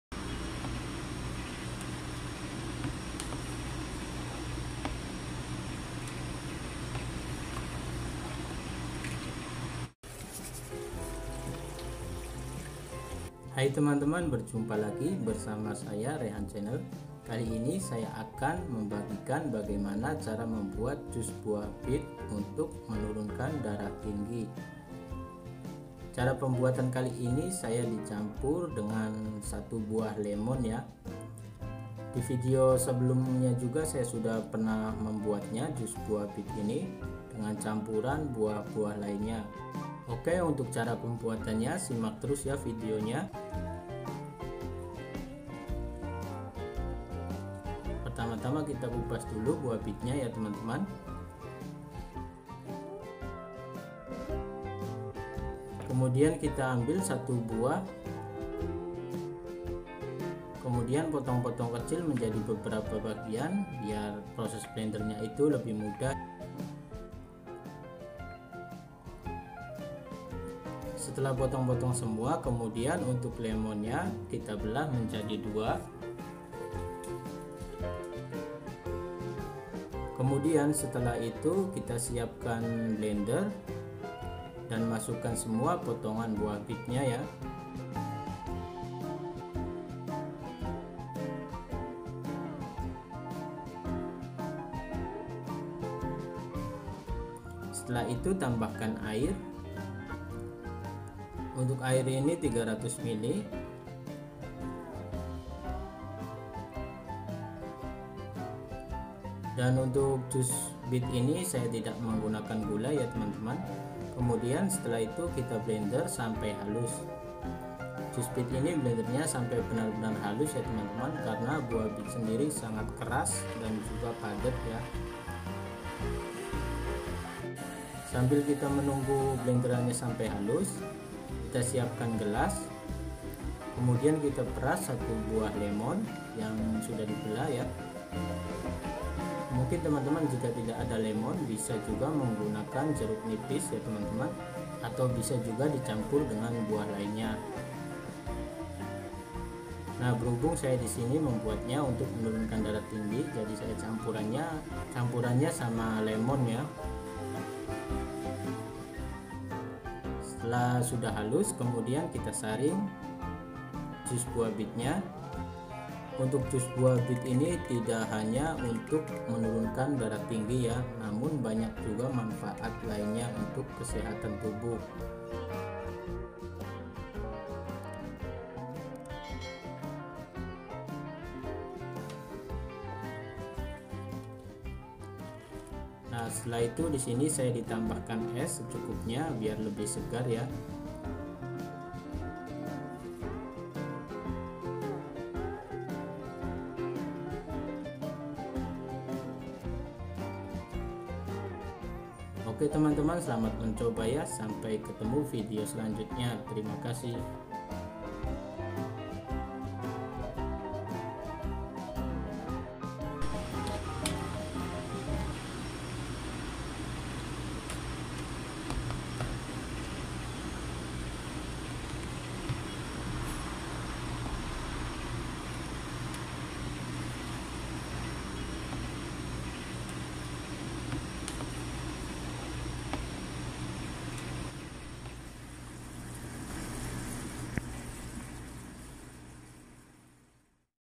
Cara Membuat Jus Buah Bit Untuk Darah Tinggi [ Jus Bit Dan Lemon ]
Buah bit memiliki banyak manfaat untuk kesehatan tubuh, salah satu untuk mengatasi tekanan darah tinggi. Buah bit juga baik dikonsumsi dengan cara dijus.

Nah, bagaimana cara membuat jus bit yang benar untuk menurunkan darah tinggi.

Jus bit dan lemon campuran ini sangat cocok rasanya mengurangi bau tanah dari buah bit dan juga lemon sendiri memiliki kandungan vitamin C yang baik.

Jus buah bit juga bisa dicampur dengan buah seperti wortel apel dan lainnya.